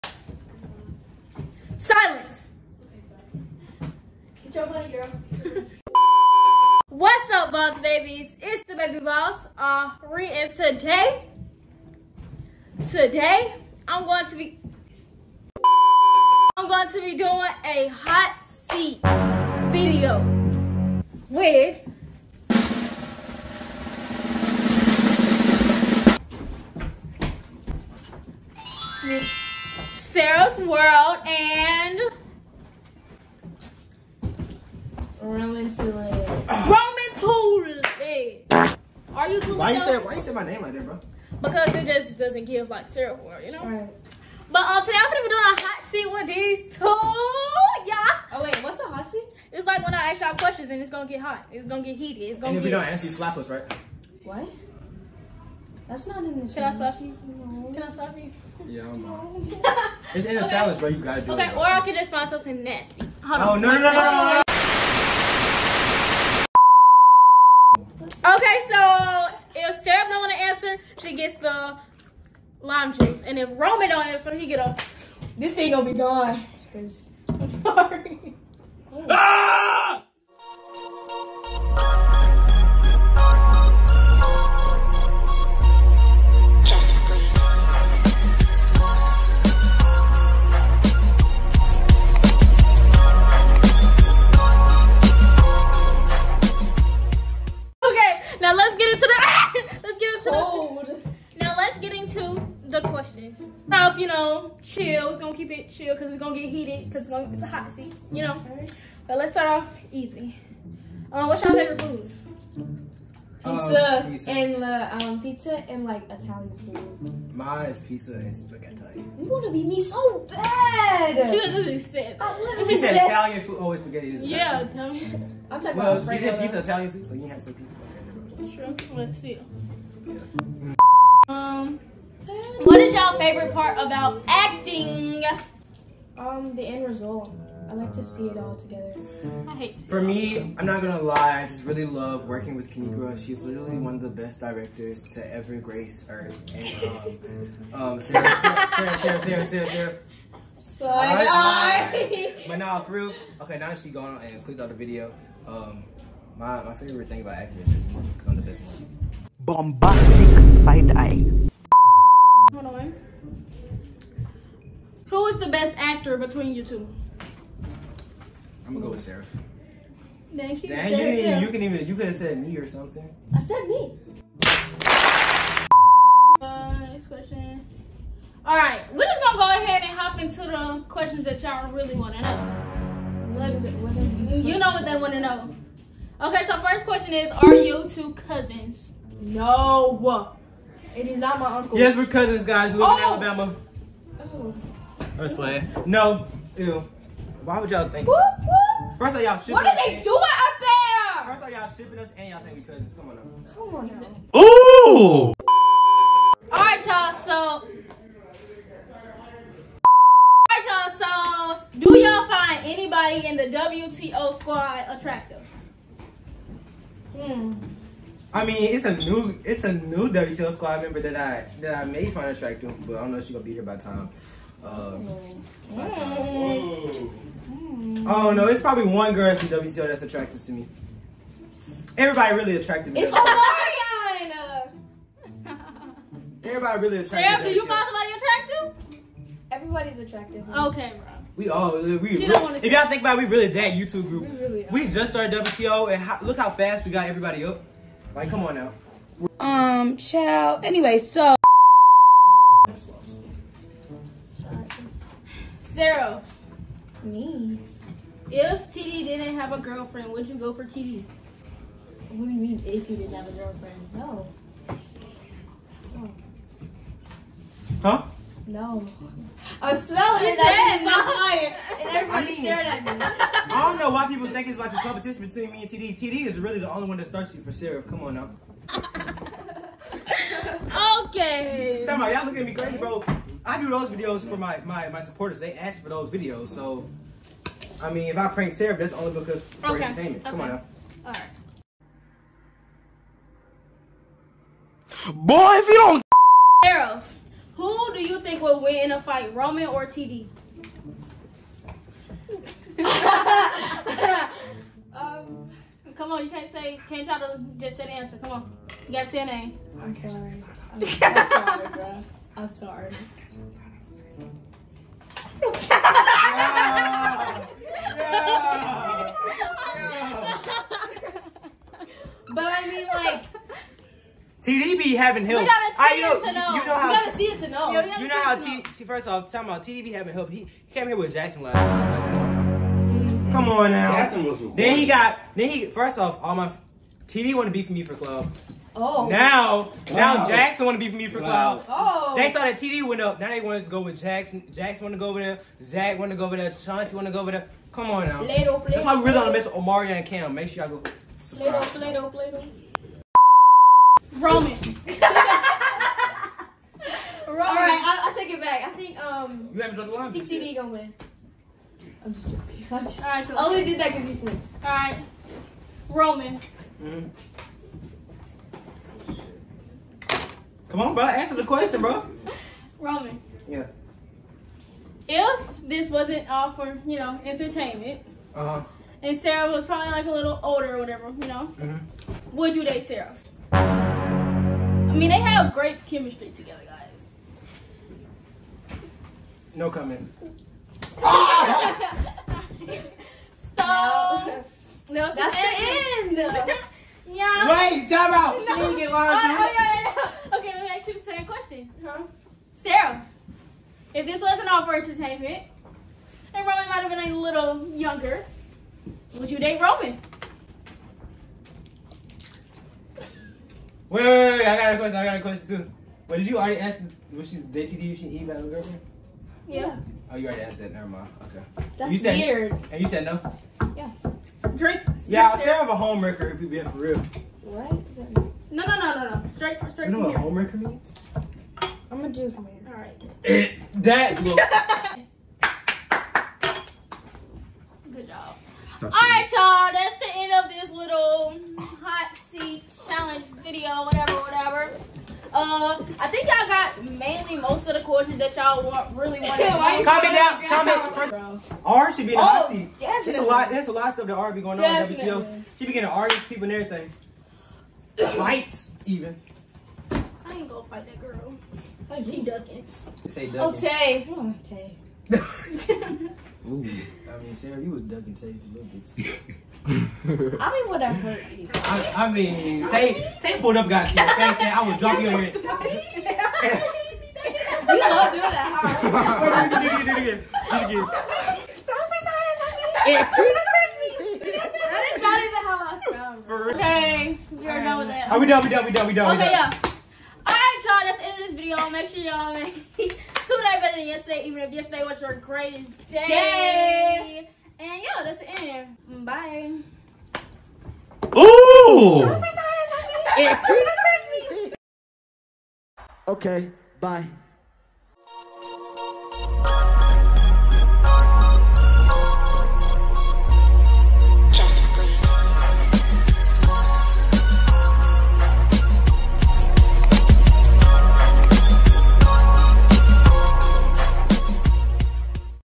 I PUT @romantoolit & @seraphsworld333 IN A HOT SEAT🔥🍵 | DO THEY LIKE EACH OTHER ?😘
CAUSE I AINT GETTING A WHOOPING FOR NO-BO-DY! 😁